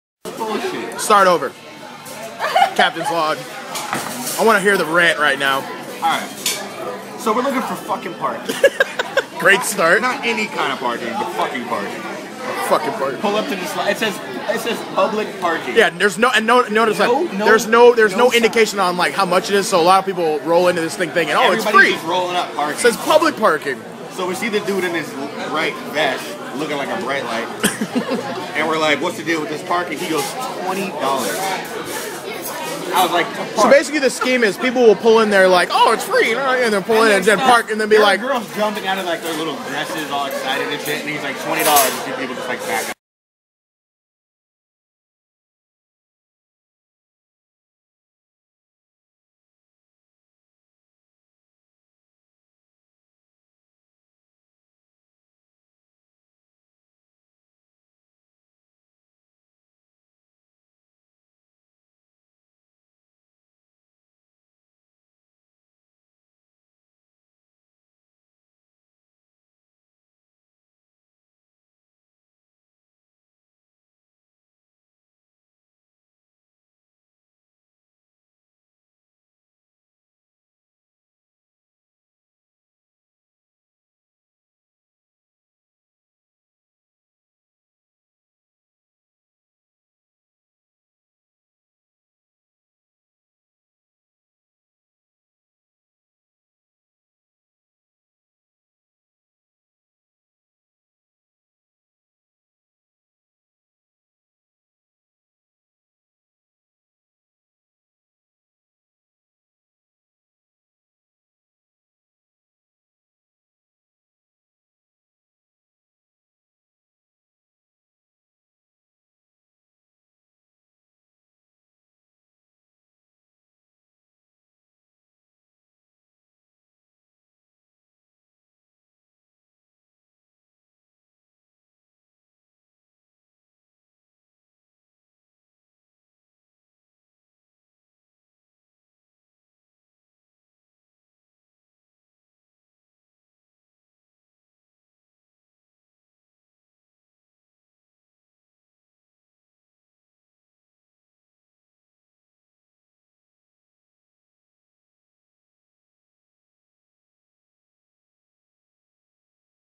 Captain's Log: Public Parking
SIGN's rant about how parking in the area blows...oh and were at Roscoes lol